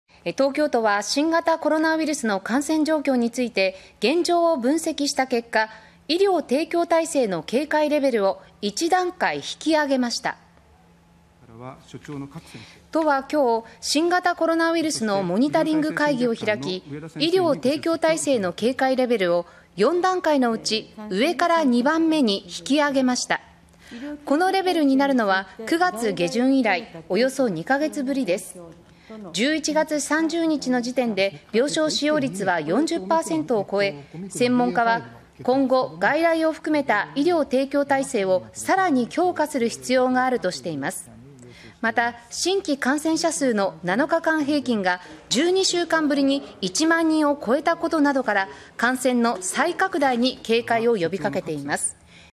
東京都の医療提供体制 警戒レベル2カ月ぶり引き上げ 病床使用率40％超(2022年12月1日)
東京都は新型コロナウイルスの感染状況について現状を分析した結果、医療提供体制の警戒レベルを1段階引き上げました。

　都は1日に新型コロナウイルスのモニタリング会議を開き、医療提供体制の警戒レベルを4段階のうち上から2番目に引き上げました。

　このレベルになるのは、9月下旬以来およそ2カ月ぶりです。

　11月30日の時点で病床使用率は40％を超え、専門家は「今後、外来を含めた医療提供体制をさらに強化する必要がある」としています。

　また、新規感染者数の7日間平均が12週間ぶりに1万人を超えたことなどから、感染の再拡大に警戒を呼び掛けています。